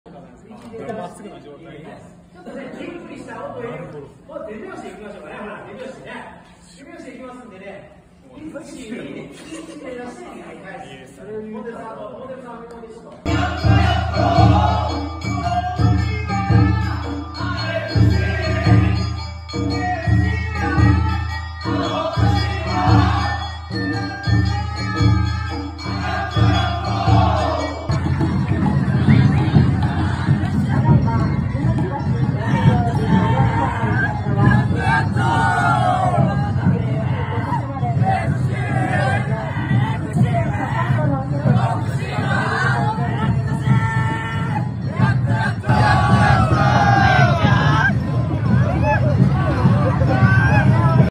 全力阿波踊り【FC徳島】
阿波踊りに全力で取り組む選手たち！